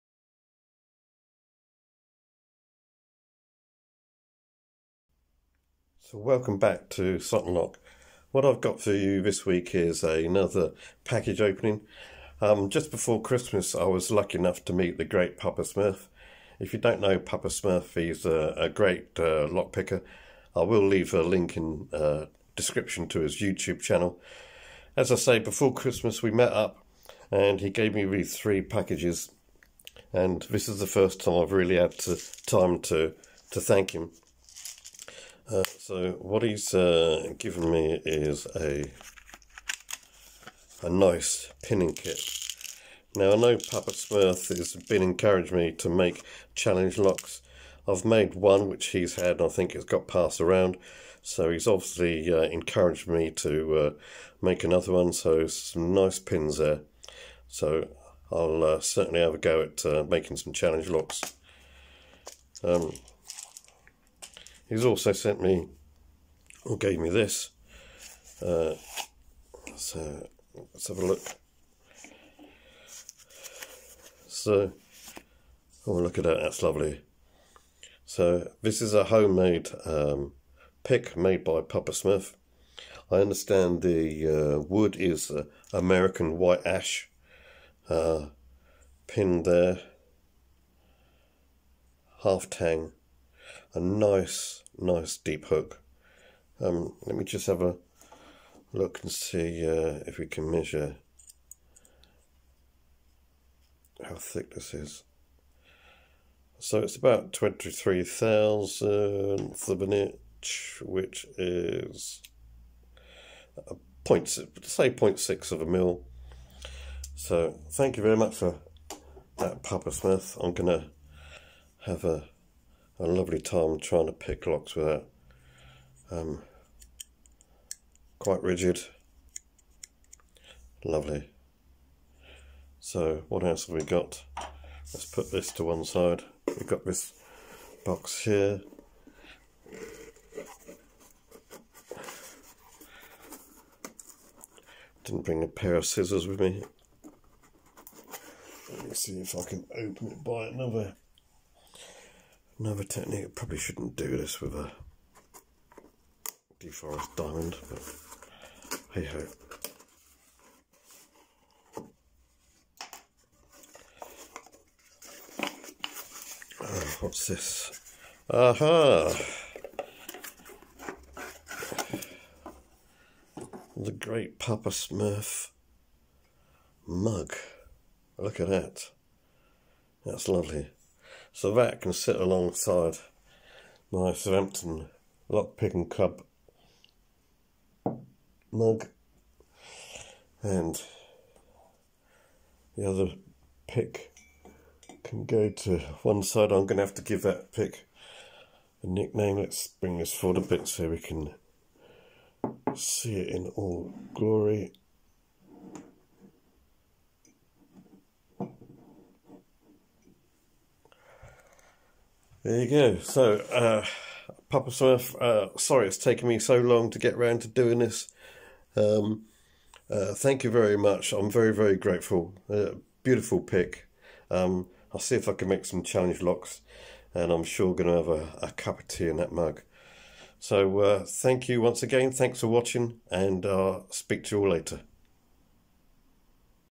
#14 Supplies, a Tool and Sustenance
I managed to meet up with a great and generous lock picker before Christmas 2021. This video is my way of thanking him for his generosity and encouragement, sorry it's taken so long to produce.

Please take the time to watch his lock picking skills at ; -